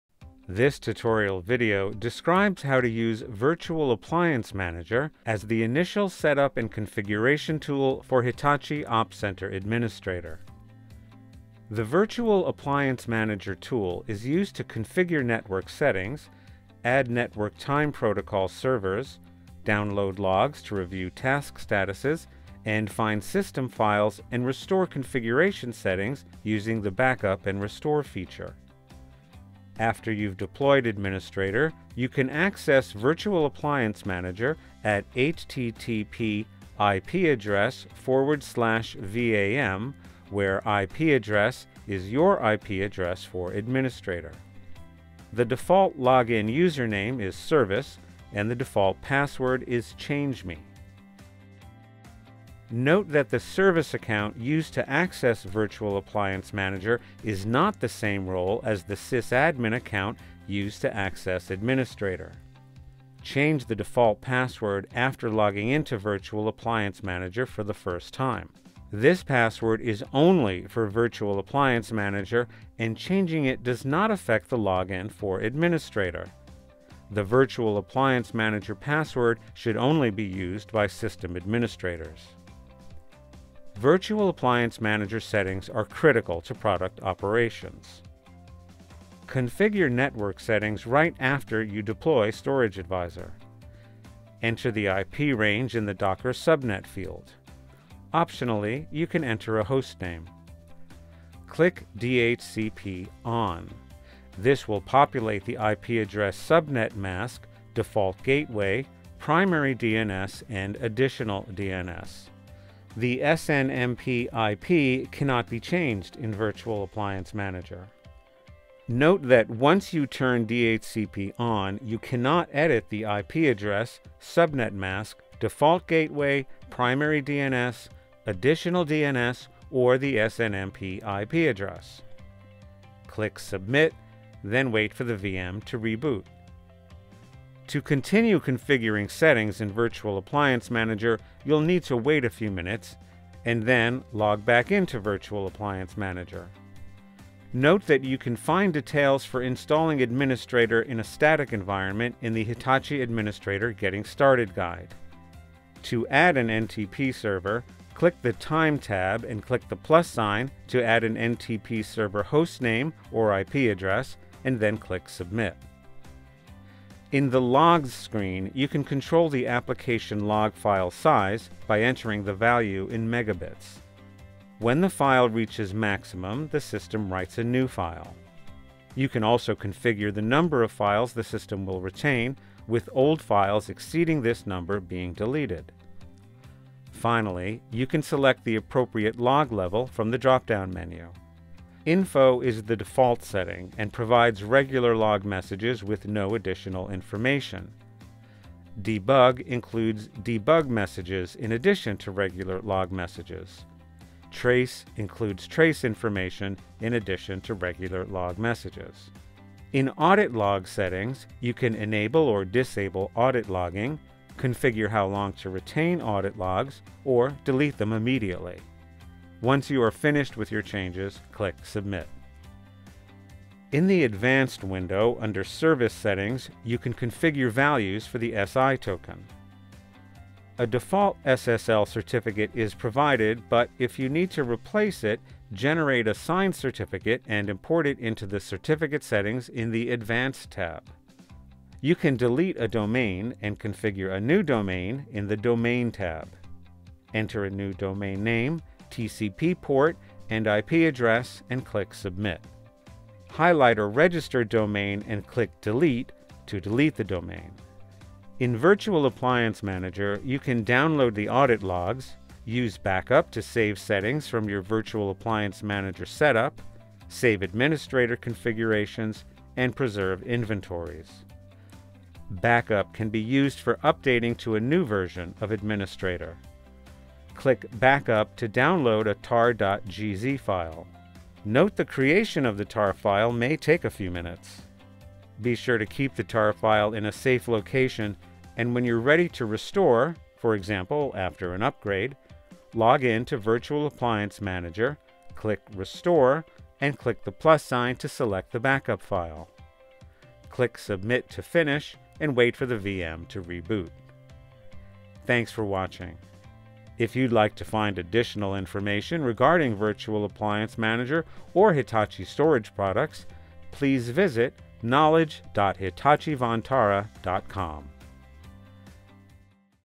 Configuring the Hitachi Ops Center Administrator Environment with the Virtual Appliance Manager Tool
This is the video which shows how to configure Hitachi Ops Center Administrator Environment with the Virtual Appliance Manager Tool